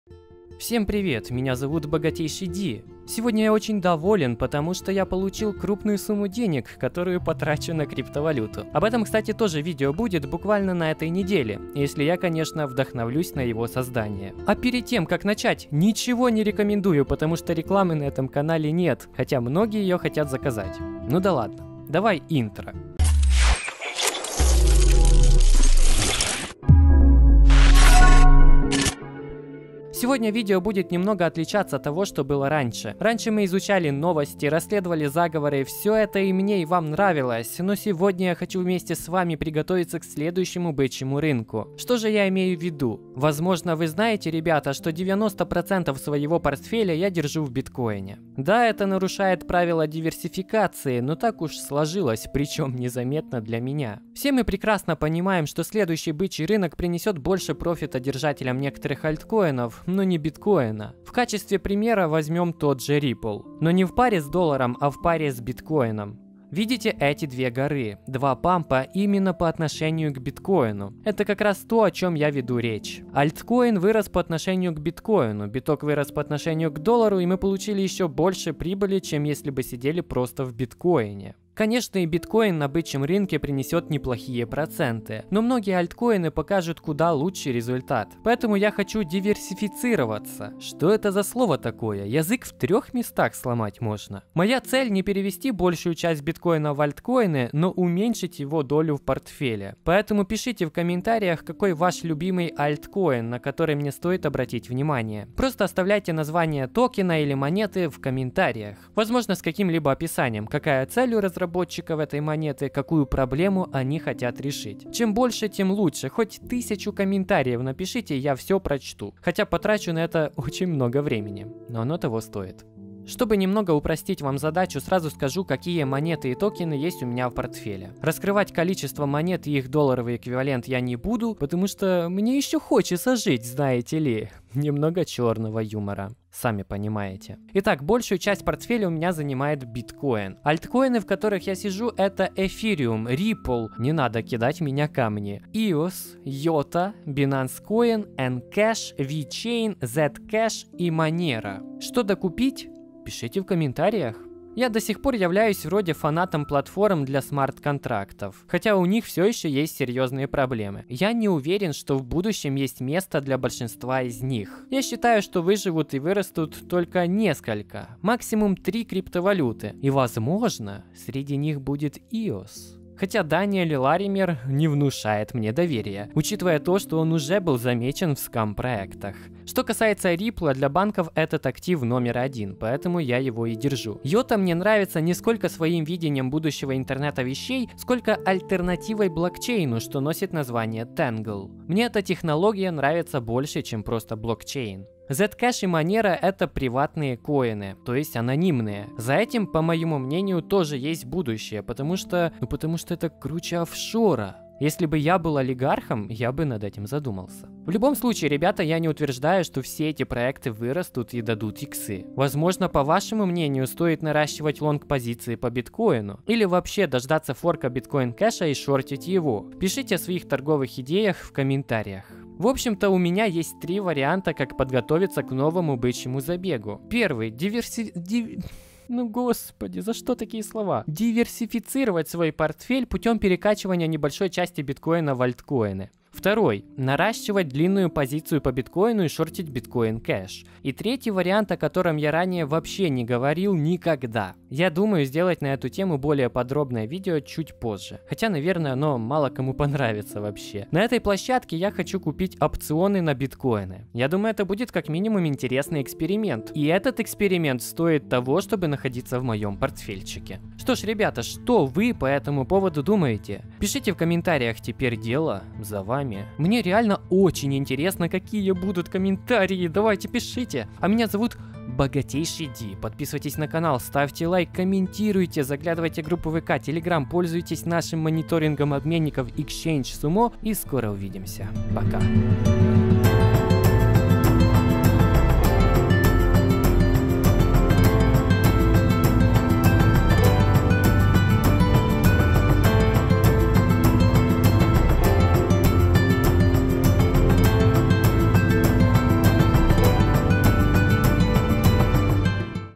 Готовимся к БИТКОИН РАЛЛИ. Какие альткоины купить? (мой криптовалютный портфель)
Готовимся к следующему бычьему рынку, когда бы он ни был! Какие альткоины купить перед Биткоин ралли? Мой криптовалютный портфель